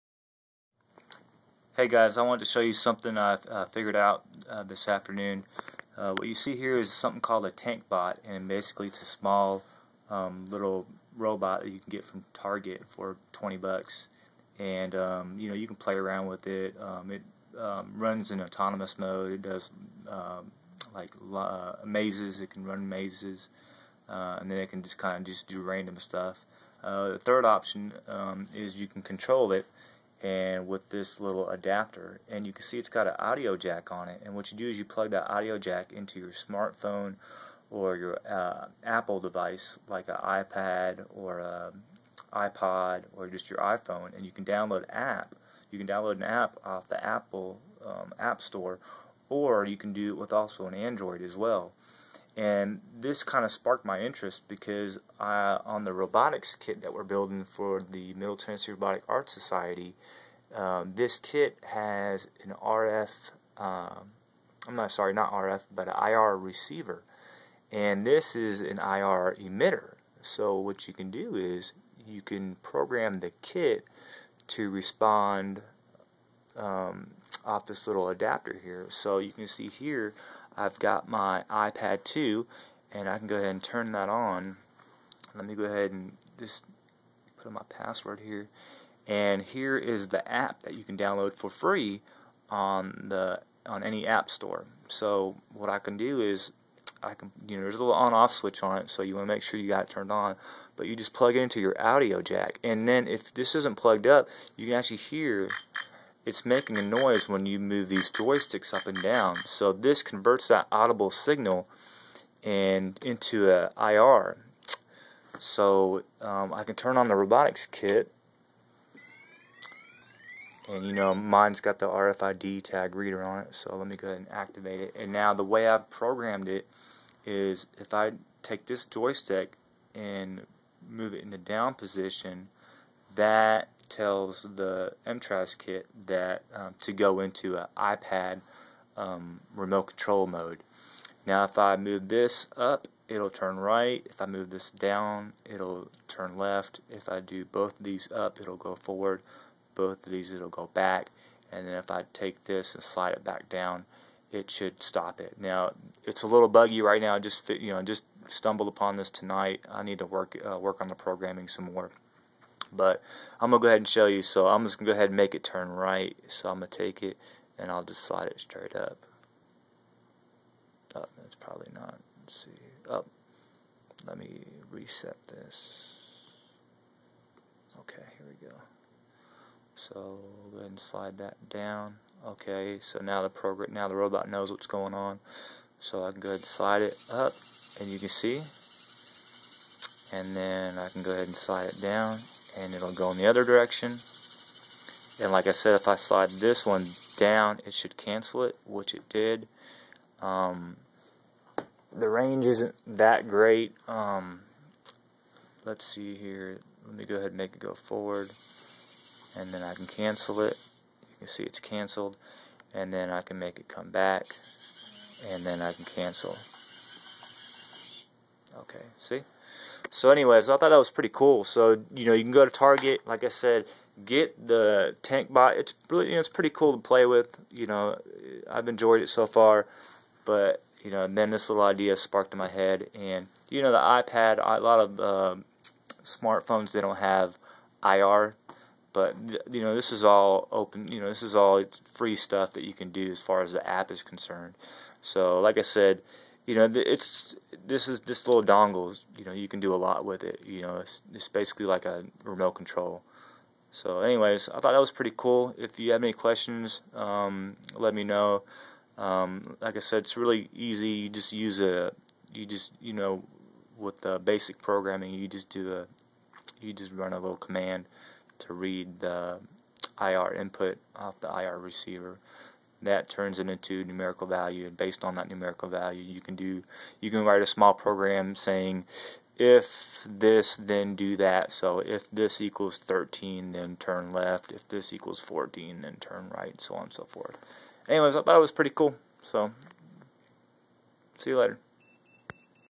MTRAS Kit Controlled via IR using IR Smartphone Adapter included in Tank Bot by desk pets
After playing around with the Tank Bot Kit by desk pets I realized I could probably read the IR output of the adapter using the MTRAS kit and then use that data to control the MTRAS kit via my iPad2 using the app originally used to the control the Tank Bot. Heres what I got so far.